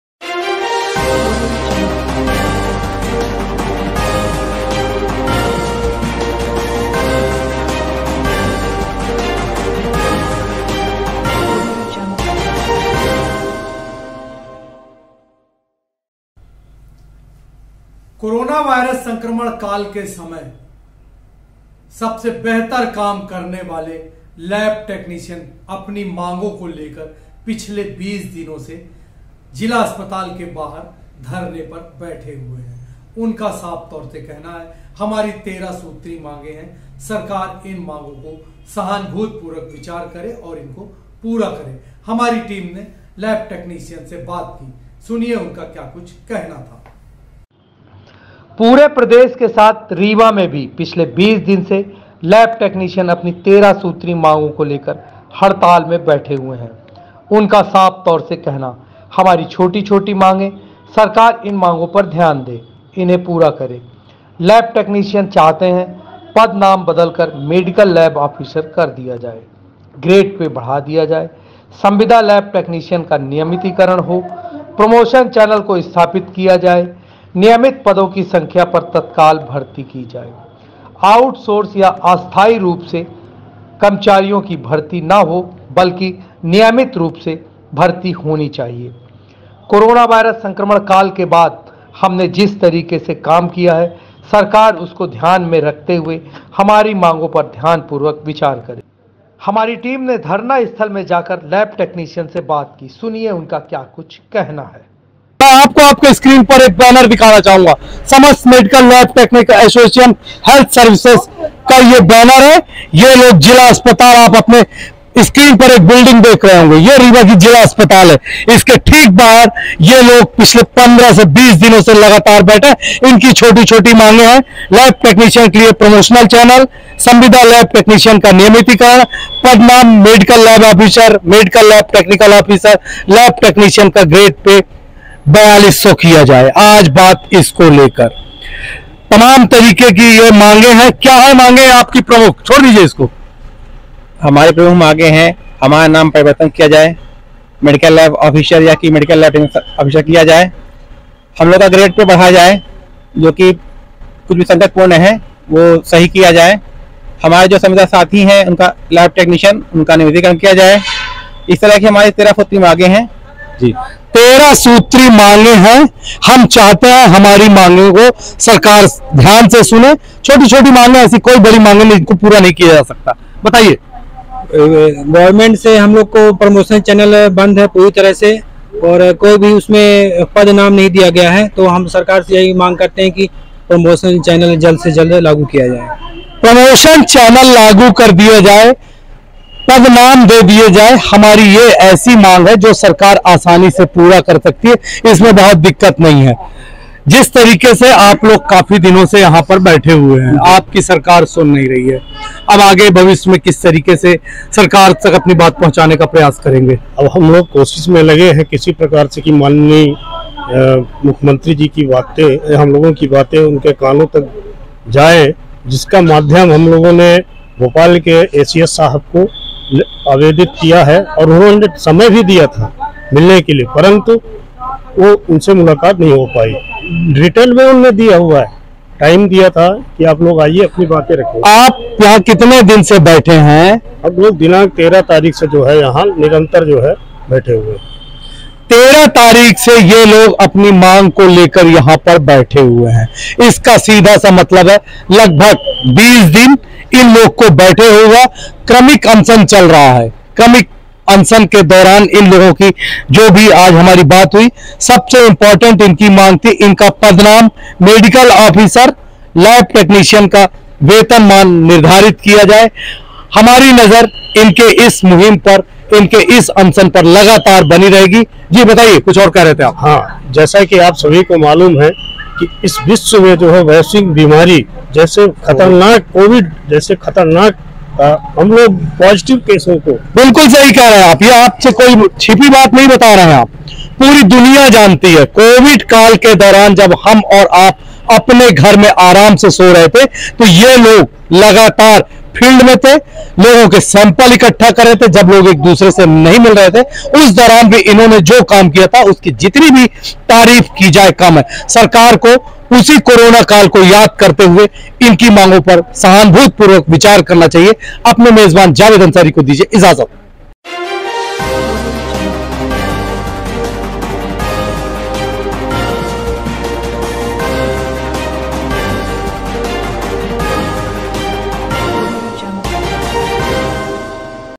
रीवा में भी पिछले 20 दिन से लैब टेक्नीशियन हड़ताल में ||NEWS NETWORK TODAY
रीवा में भी पिछले 20 दिन से लैब टेक्नीशियन  हड़ताल में